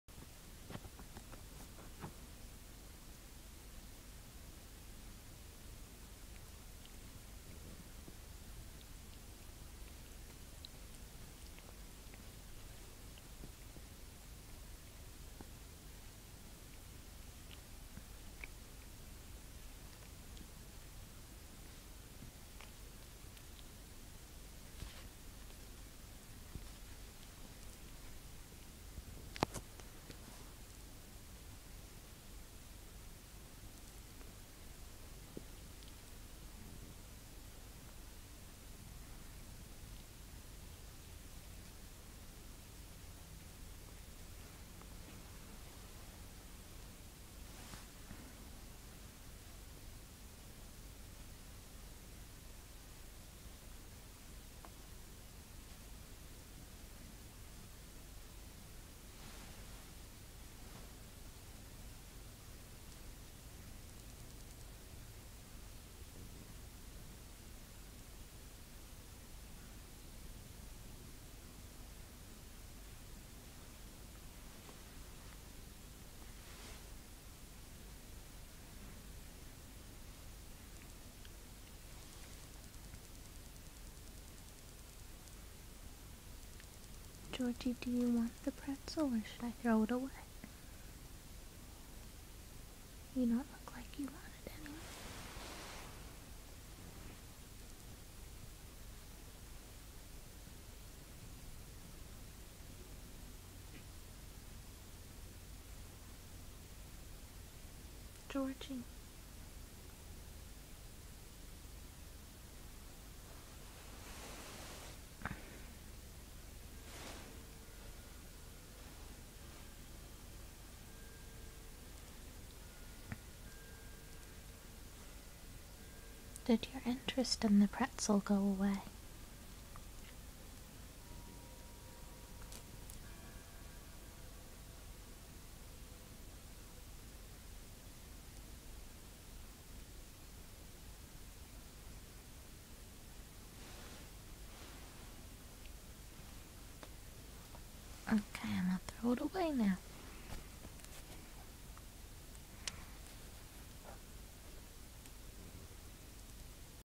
George's First Pretzel- Part 2
George checks out the pretzel again but decides he doesn't want it after all.